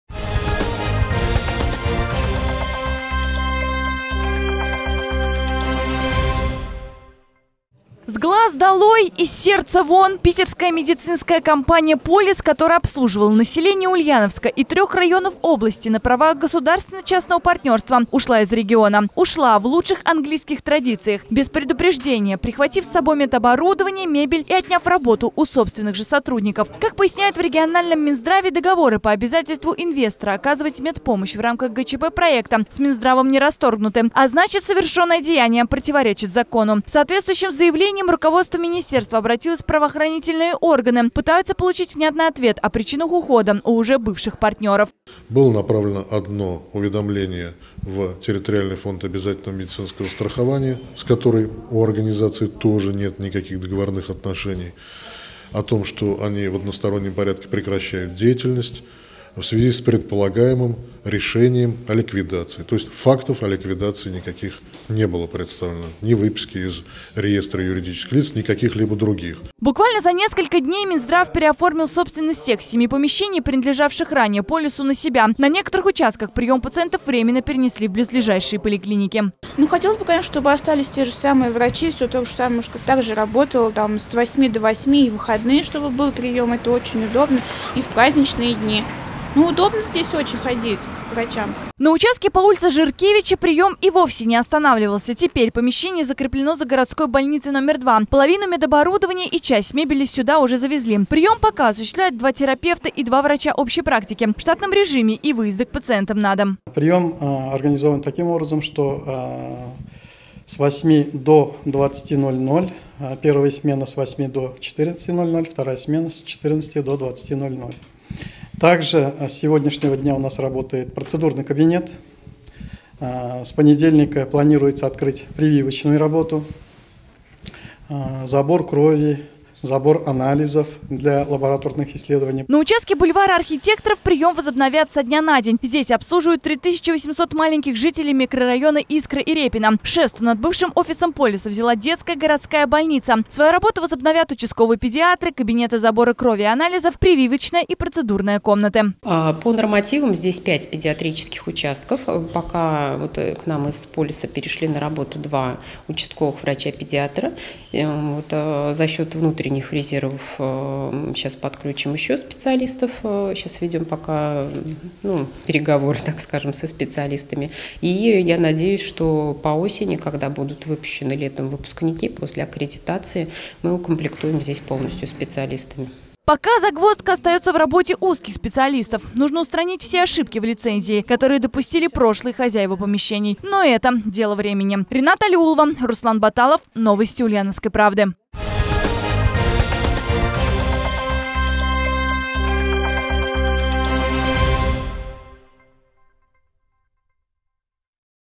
Ушли не попрощавшись. Кто примет ульяновцев вместо врачей «Полиса»
Медицинская компания «Полис» в одностороннем порядке  свернула деятельность, оставив после себя лишь голые стены помещений. Кто теперь будет лечить детей и взрослых? Выясняла Рената Алиулова.